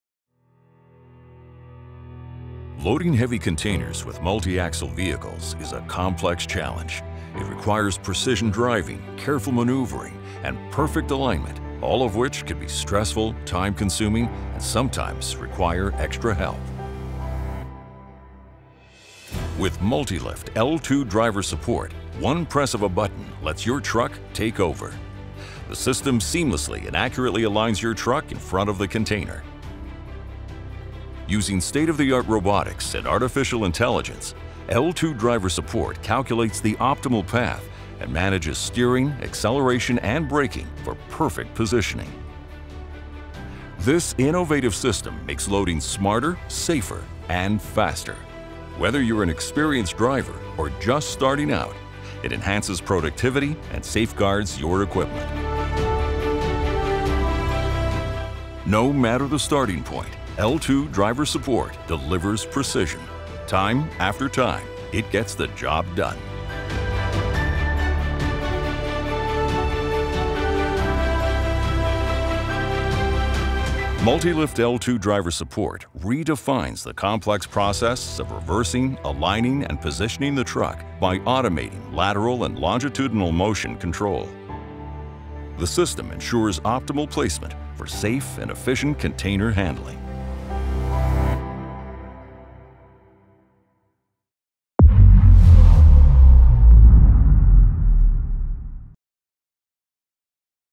Smarter Loading With L2 Driver Support for the Hiab's MULTILIFT products
Hiab’s L2 Driver Support is an assistance feature that reverses the ego-vehicle into an optimal position from where the driver can swiftly pick up or drop off a container/body with Hiab’s MULTILIFT products.